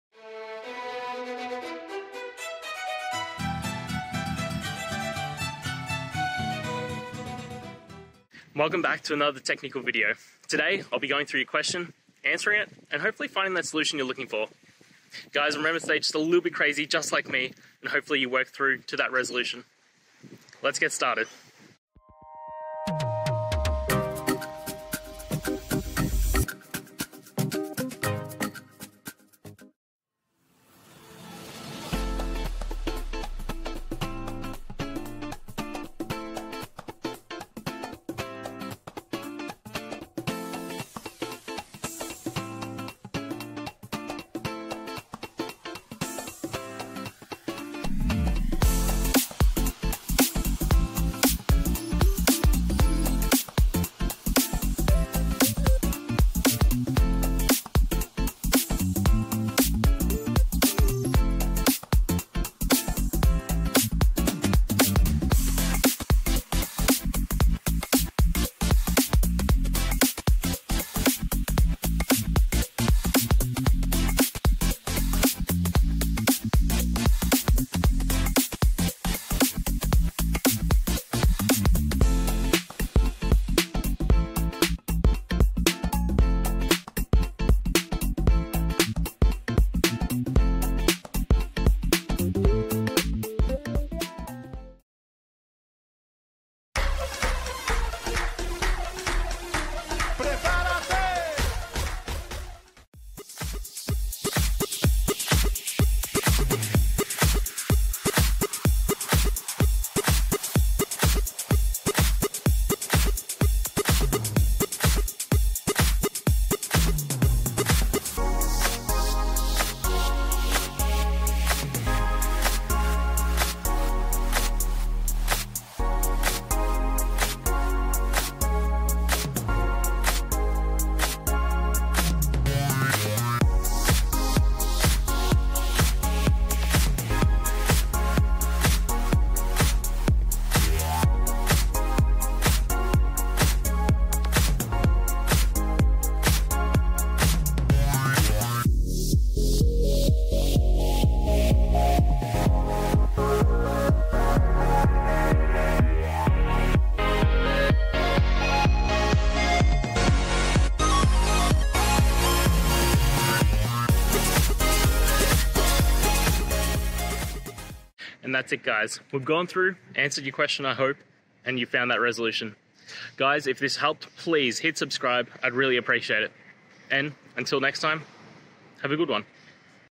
Numba Caching Issue: Fix 'Cannot Cache Function' Error and Locator Problems
In this video, we dive into a common challenge faced by developers using Numba: the 'Cannot Cache Function' error and related locator issues. We'll explore the underlying causes of these problems and provide step-by-step solutions to help you effectively resolve them. Whether you're a seasoned programmer or just starting with Numba, this guide will equip you with the knowledge to enhance your coding experience and optimize your performance.

Today's Topic: Numba Caching Issue: Fix 'Cannot Cache Function' Error and Locator Problems

Related to